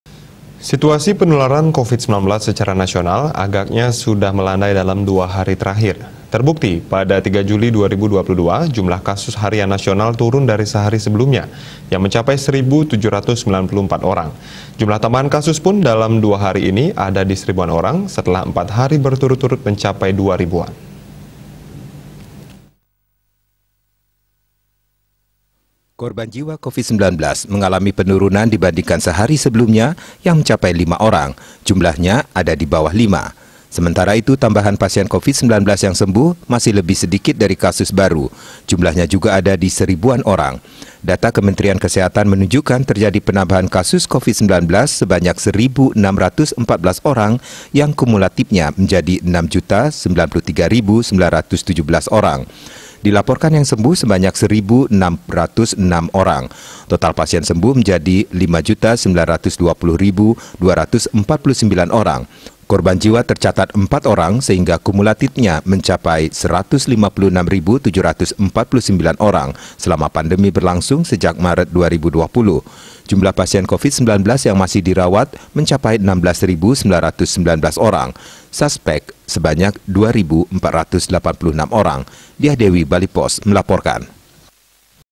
SUDAH MELANDAI, NASIONAL KEMBALI CATAT SERIBUAN KASUS COVID 19 BARU PER 3 JULI
SITUASI PENULARAN COVID-19 SECARA NASIONAL AGAKNYA SUDAH MELANDAI DALAM 2 HARI TERAKHIR. TERBUKTI, PADA 3 JULI 2022, JUMLAH KASUS HARIAN NASIONAL TURUN DARI SEHARI SEBELUMNYA YANG MENCAPAI 1.794 ORANG. JUMLAH TAMBAHAN KASUS PUN DALAM DUA HARI INI ADA DI SERIBUAN ORANG, SETELAH 4 HARI BERTURUT-TURUT MENCAPAI DUA RIBUAN.